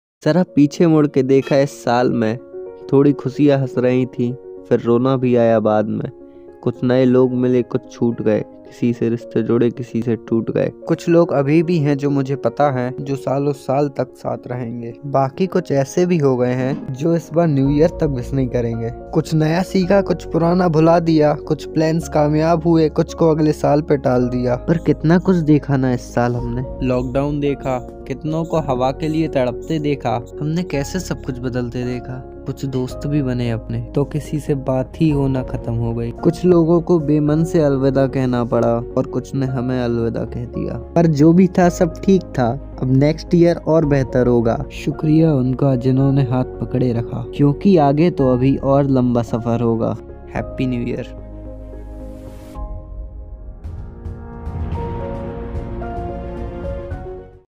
Happy New Year yaar - poetry by curious thinker
This poetry is written by Abhash Jha & voice over by aadi jain.

                            ‘ curious thinker ’